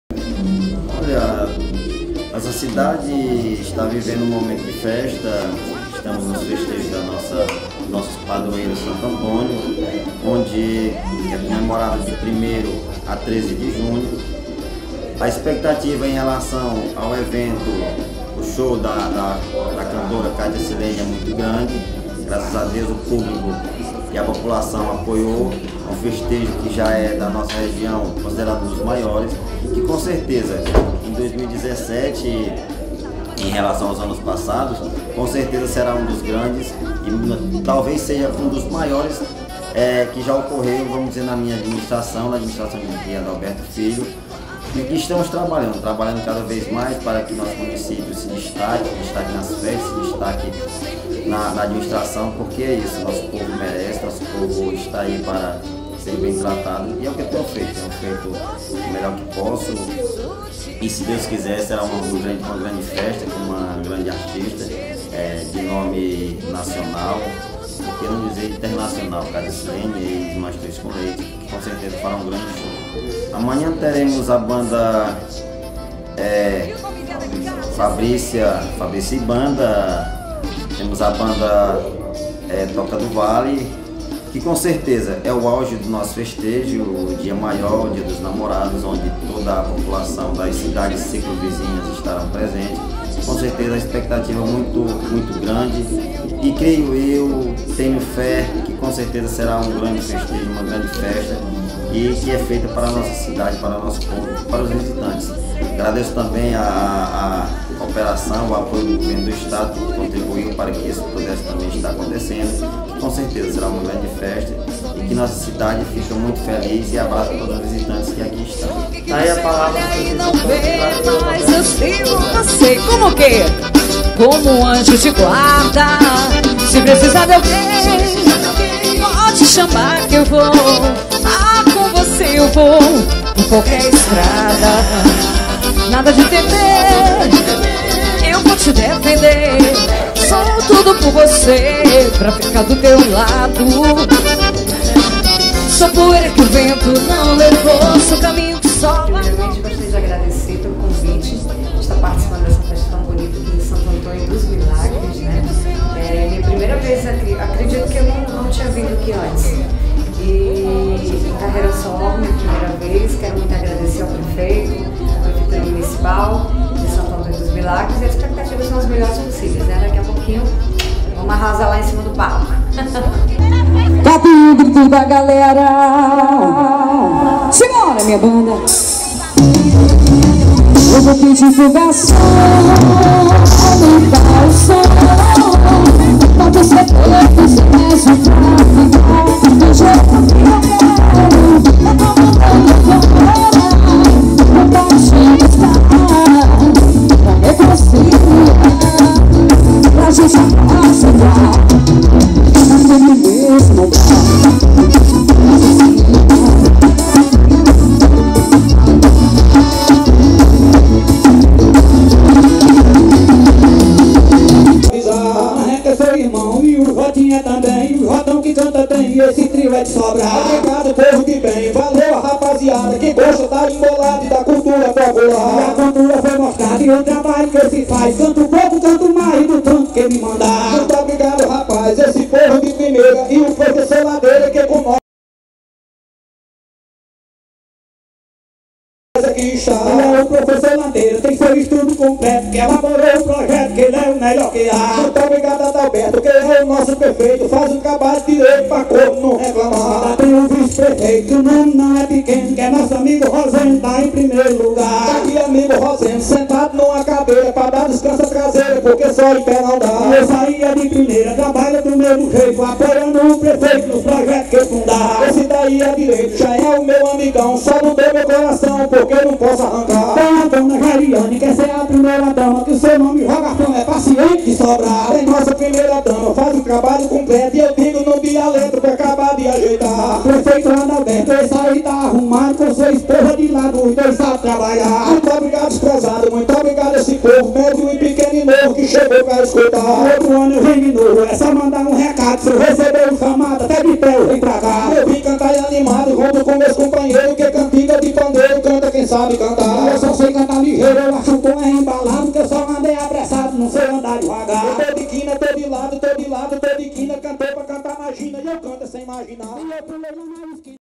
Segundo dia de festa em Santo Antônio dos Milagres - PI 2017
COMPARTILHE E CURTA NOSSO CANAL. DA ESSA MORALZINHA POR FAVOR. O DIA 11 de junho de 2017, foi marcado na cidade de Santo Antônio dos Milagres - PI, por grande apresentação dos repentistas, Jota, Jotão e Jotinha, além de grande festa em Praça pública com a musa do forró romântico Katia Cilene. Também nos bastidores entrevista com o prefeito Dr. Adalberto Filho e Katia Cilene.